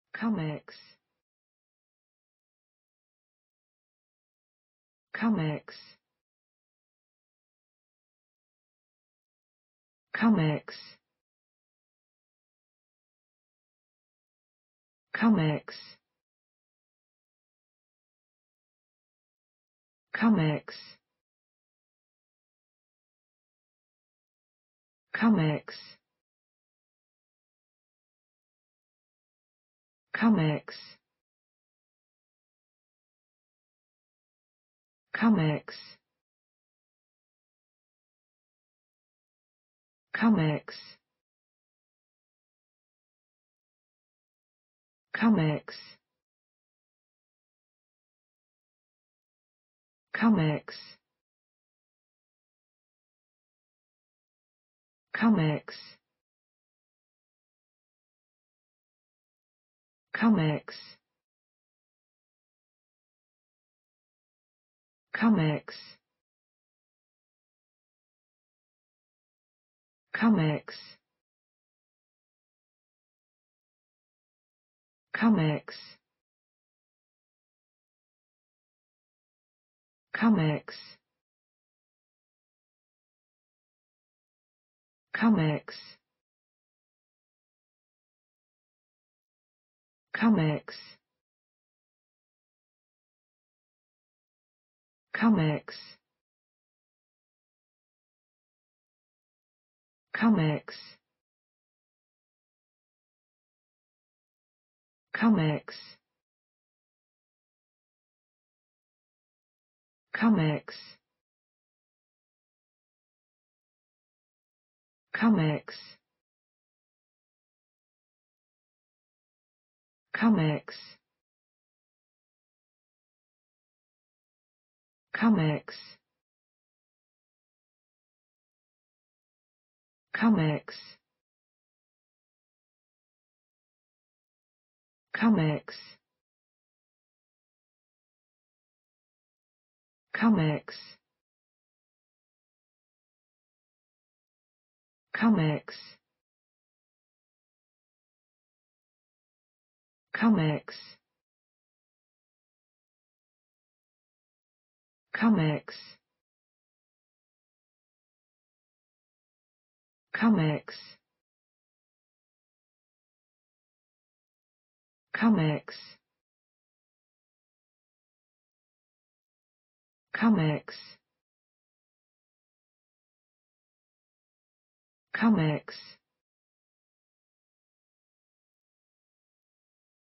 how to pronounce the english word Comex.
Follow me!Learn from me how to pronounce the english word Comex.This video shows you how to pronounce the English word  Comex .